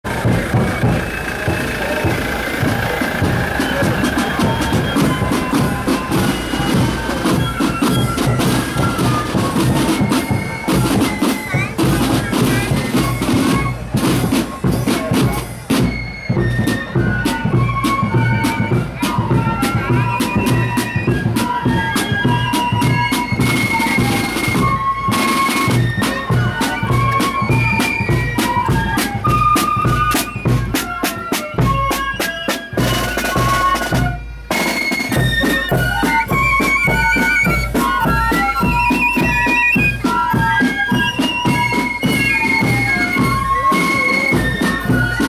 Articlave Flute Band @ Black Saturday 2015 (3)
This year the Black Saturday is in Portstewart Co. Londonderry.